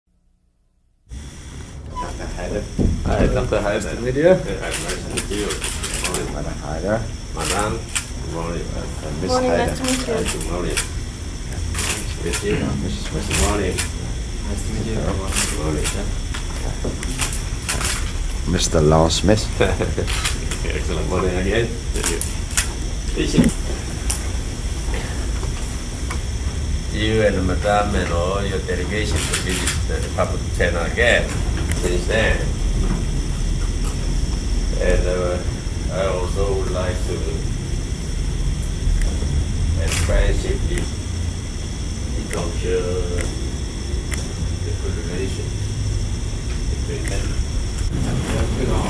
1998年05月05日蕭萬長院長接見奧地利自由黨黨魁海穗博士
1998年05月05日蕭萬長院長接見奧地利自由黨黨魁海穗博士